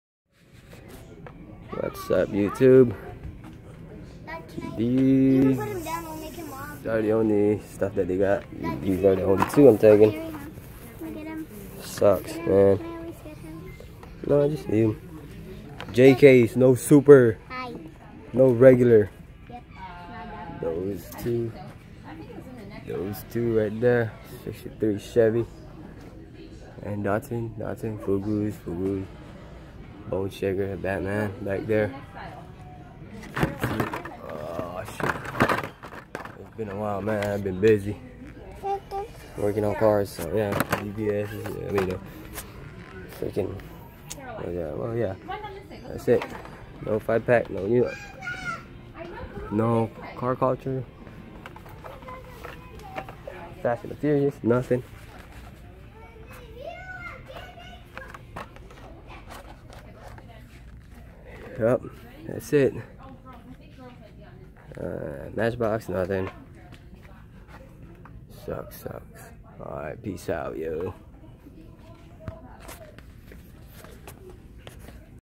Hot Wheels hunting @ target.
Finally some new ones only 1 of my local target put up new case..